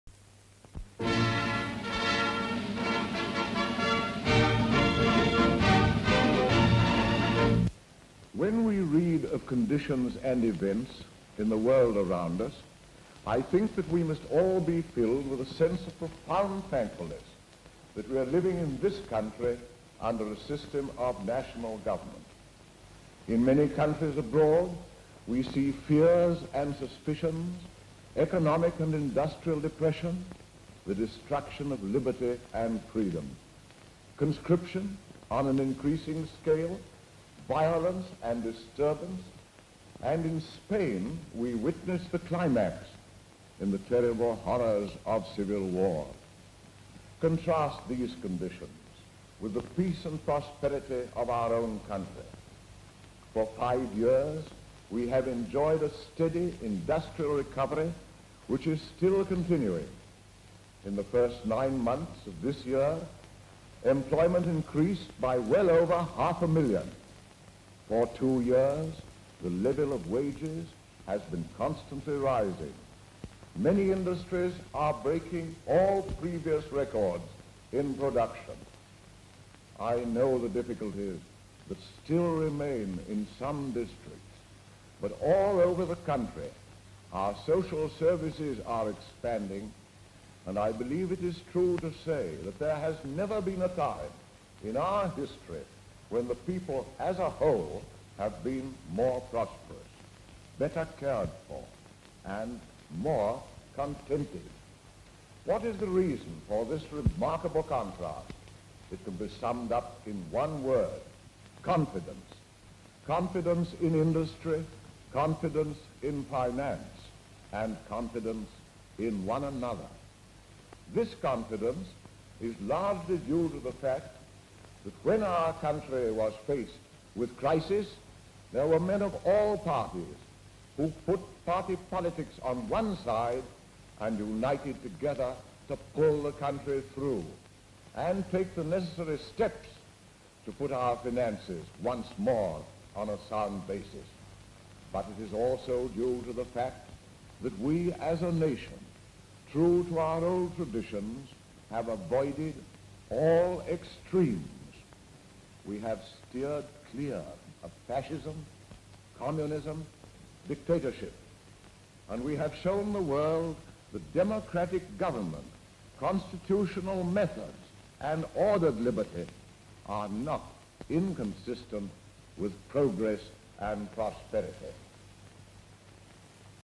Prime Minister Baldwin delivers address on recovery efforts (1936)
Prime Minister Stanley Baldwin reports on recovery efforts fueled by consumer confidence and party cooperation

Full Description:

SLATE INFORMATION: The Prime Minister Speaks Out

ENGLAND: Unknown
INT

BALDWIN, Stanley (Earl) Speaks on Britain's Recovery & Trade Prosperity, compared with other Countries.

interwar years, 1st Earl Baldwin of Bewdley, economy, speech, speeches
Background: Prime Minister Stanley Baldwin reports on recovery efforts fueled by consumer confidence and party cooperation